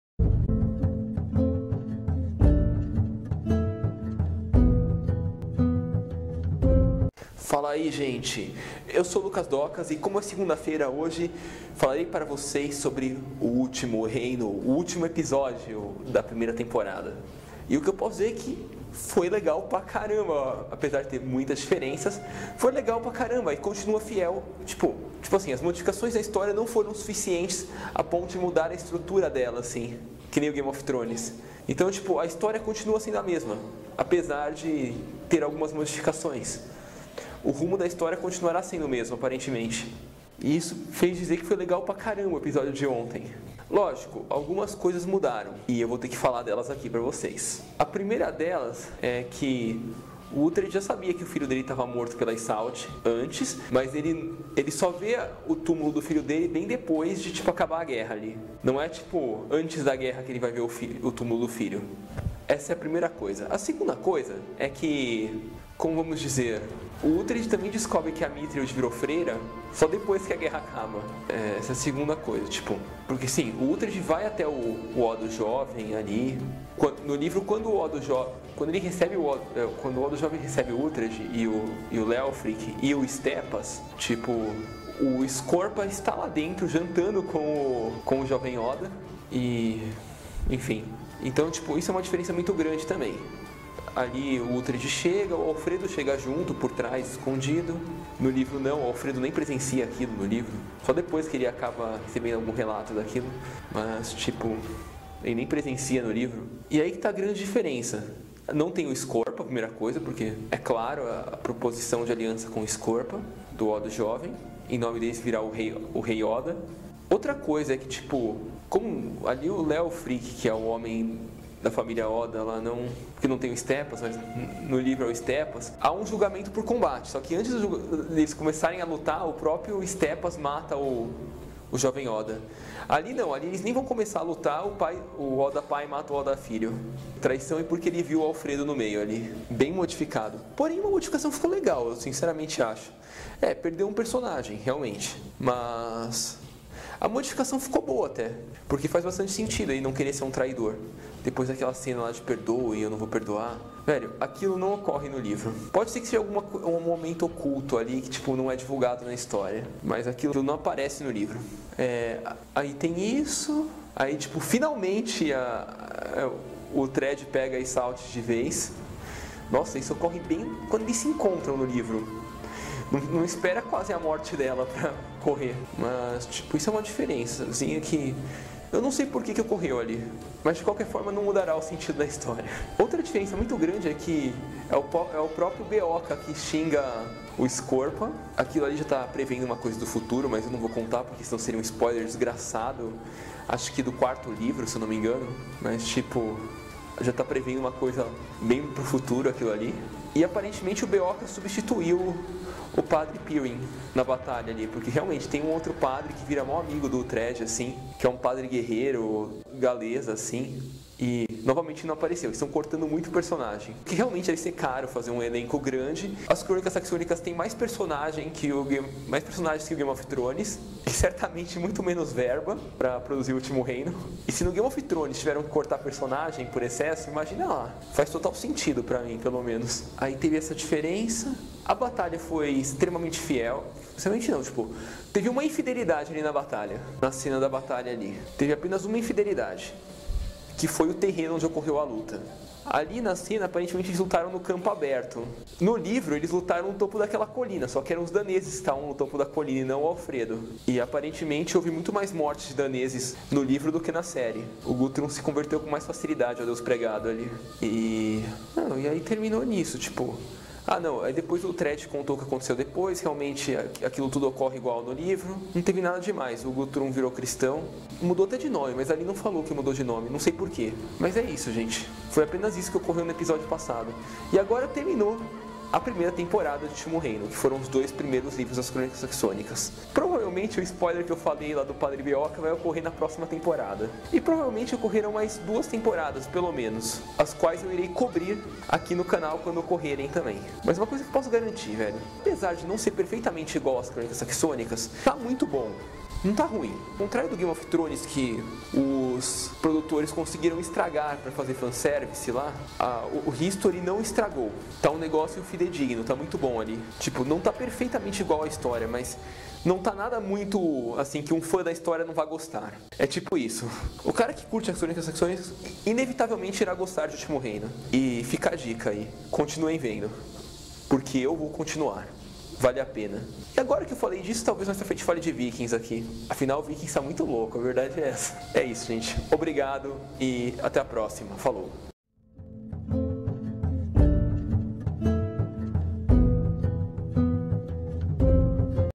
Sobre o 10° episódio de O Último Reino
Neste vídeo eu falo sobre o último episódio da primeira temporada da série O Último Reino.

Vale muito apena continuar acompanhando!